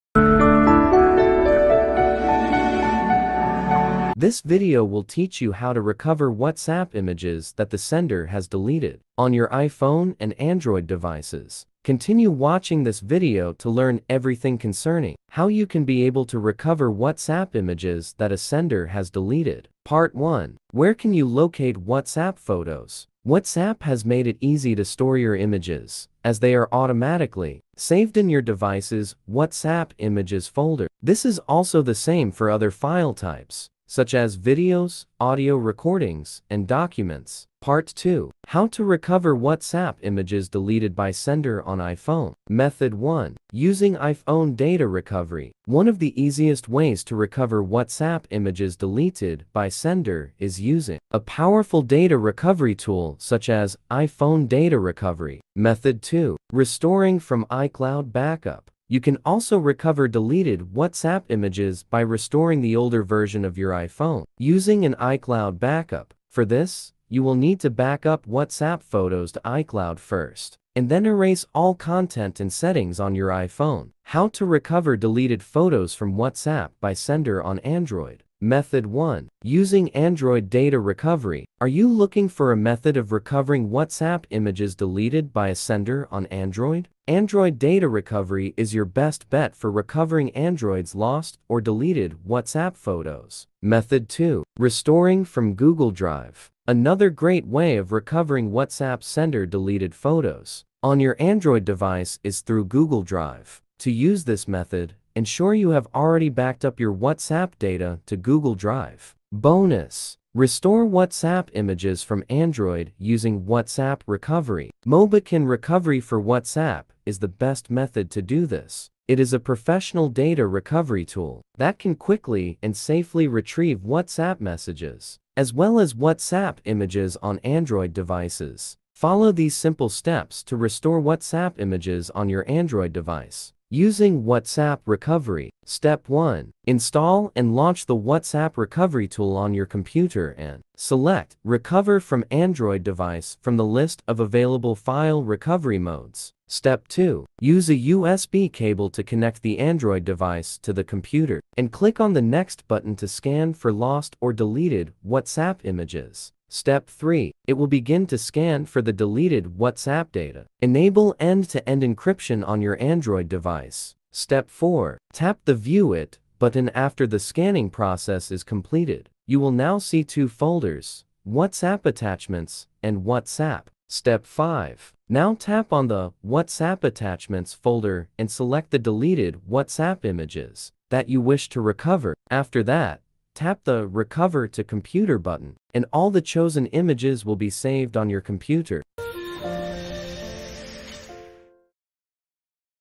How to Recover WhatsApp Images Deleted by Sender
Looking for a way on how to recover WhatsApp images deleted by sender on Android and iPhone? We have explained several solutions in this video. You will find that WhatsApp delete photo recovery is as easy as pie.